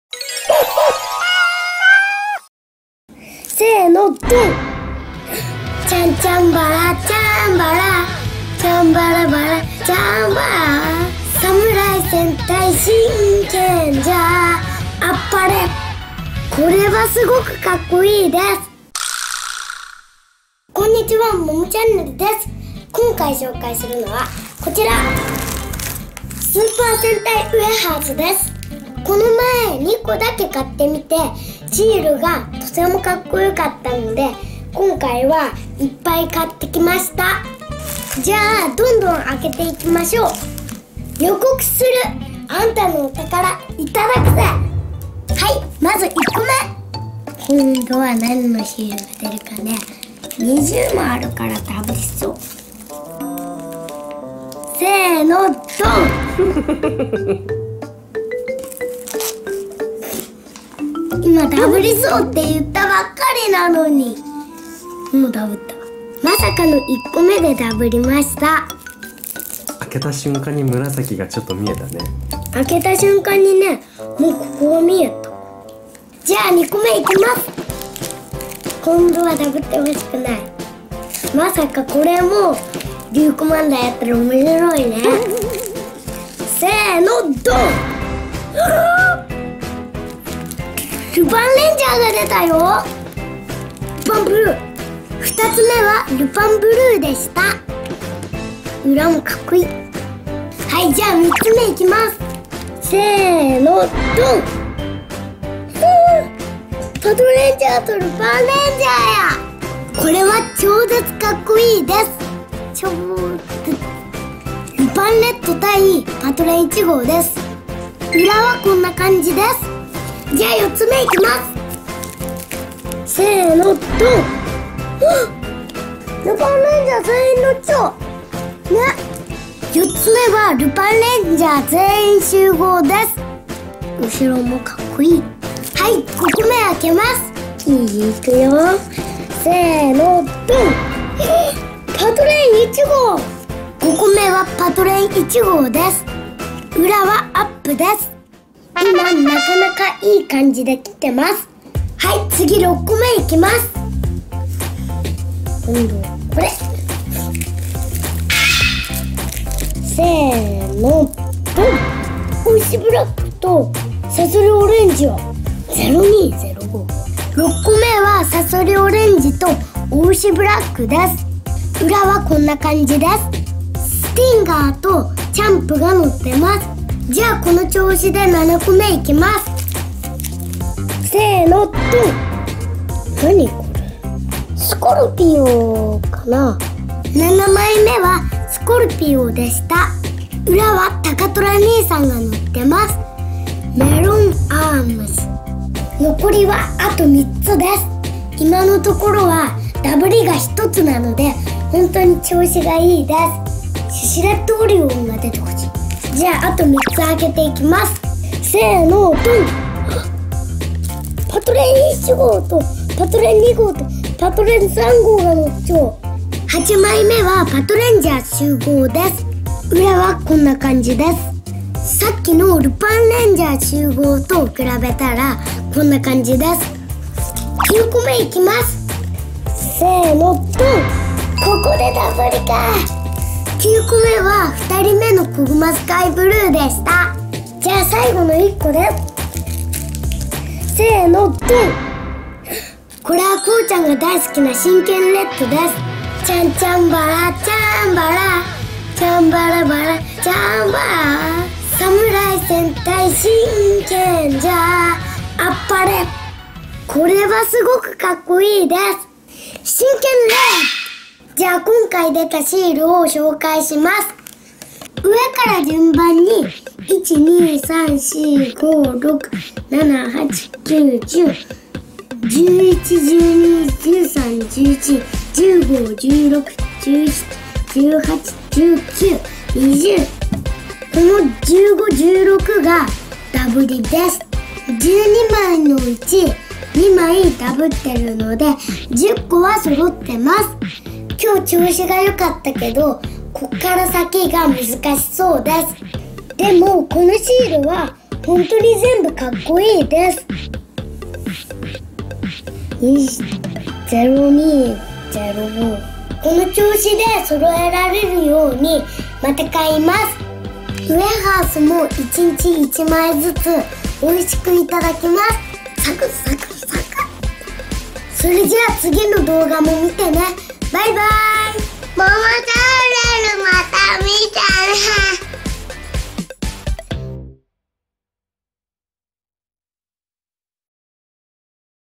ルパンレンジャーVSパトレンジャー スーパー戦隊ウエハース 10個開封☆Lupinranger VS Patoranger Snack☆モモちゃんねる☆☆
いつもコーキtvをご家族でご視聴下さり有難う御座います！今後も親子で一緒に楽しめる動画をお届けできるよう頑張ります！ スーパー戦隊シールがあまりにもかっこいいので、10個買いました＾ー＾☆
【こうちゃんの変身・なりきり動画】
モモちゃんねるまた見てね☆